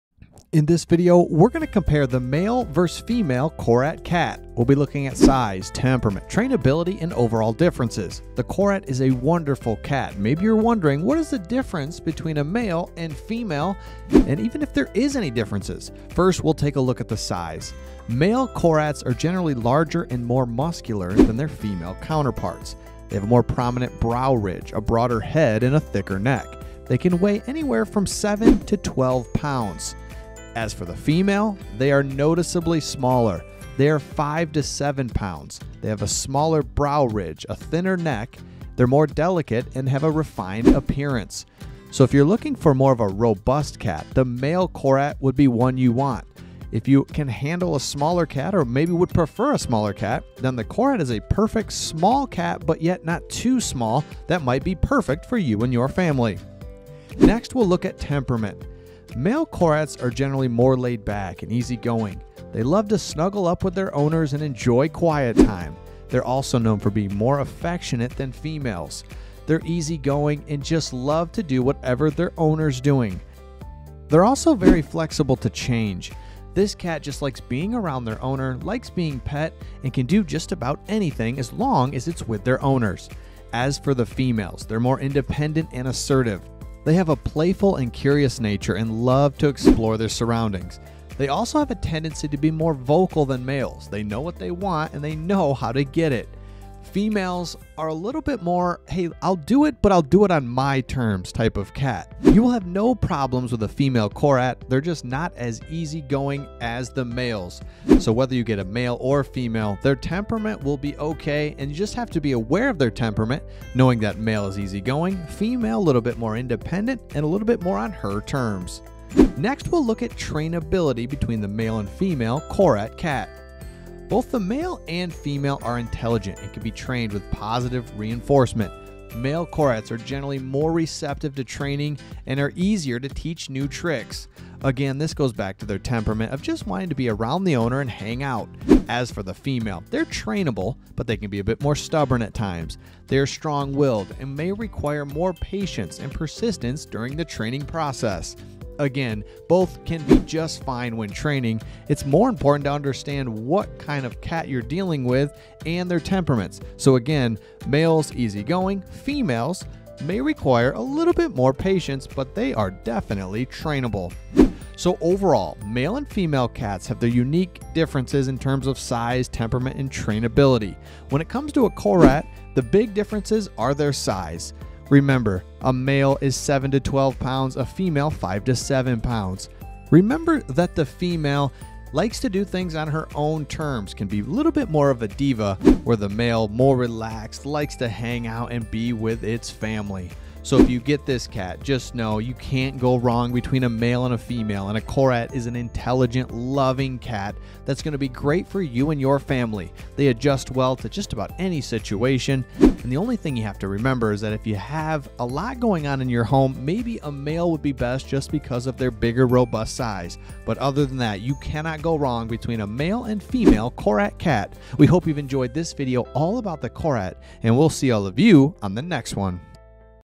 Male Korat vs Female Korat Cat - Compare and Contrast
Male Korat vs Female Korat Cat - Compare and Contrast. What is the difference between the male and female korat cat? Male and female korat cat size temperament trainability.

korat
korat cat
male korat
female korat